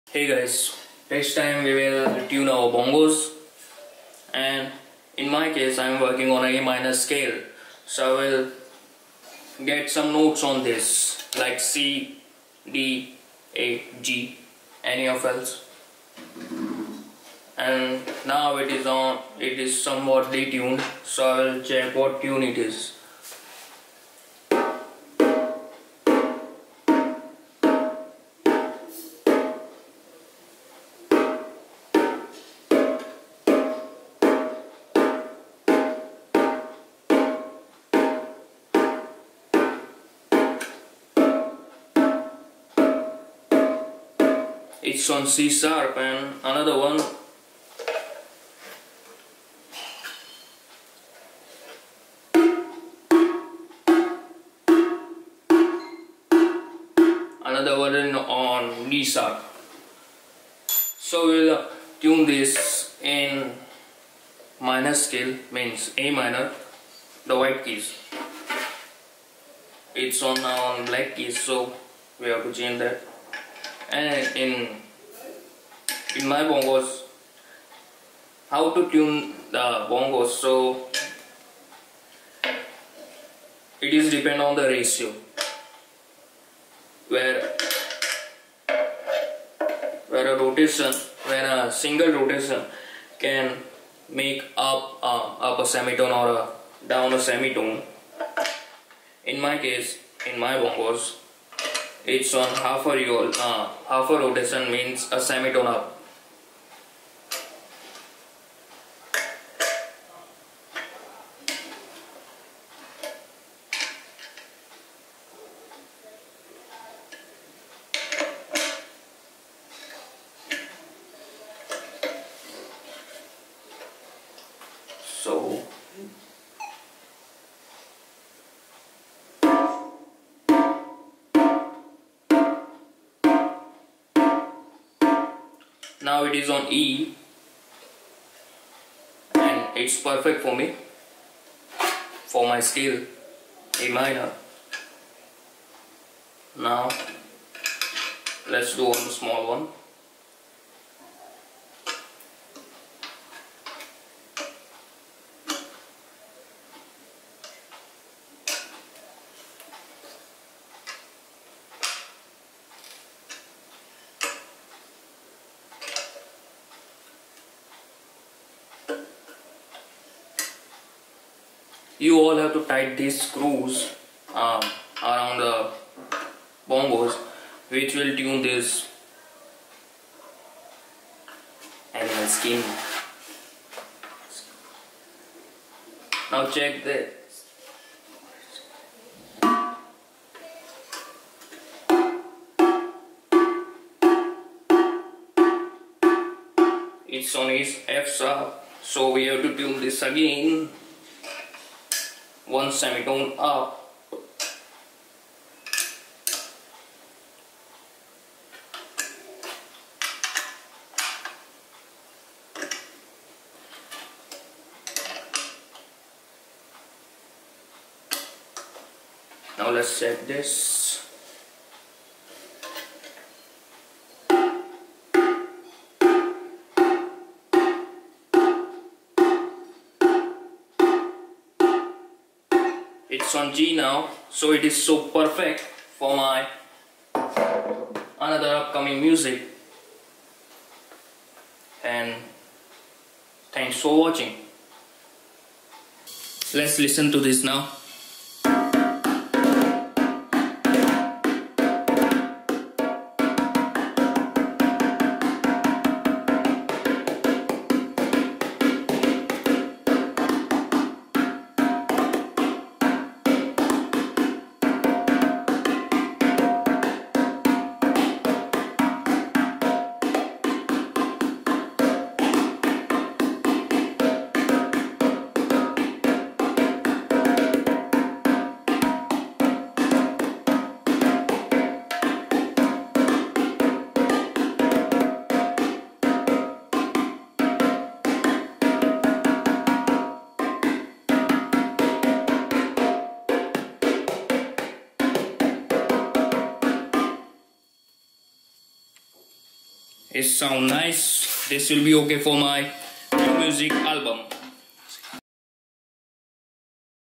How to tune your bongos [percussions]
Here is how to tune your bongos it's just a simple tutorial.
I am tuning this because it will be used in my new music album.